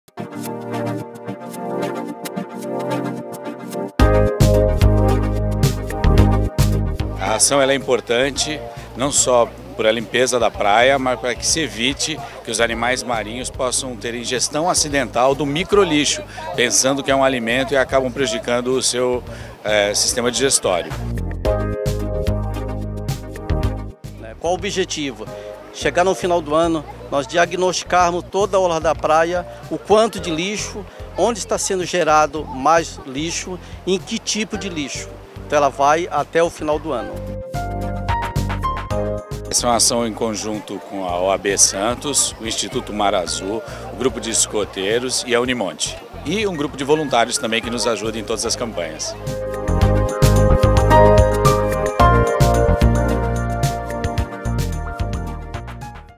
Voluntários de diversas entidades participam de mutirão de limpeza da praia em Santos
Cerca de 60 pessoas participaram da ação, que recolheu mais de 17 quilos de resíduos da faixa de areia entre o Gonzaga e a altura do Aquário, na Ponta da Praia - saiba mais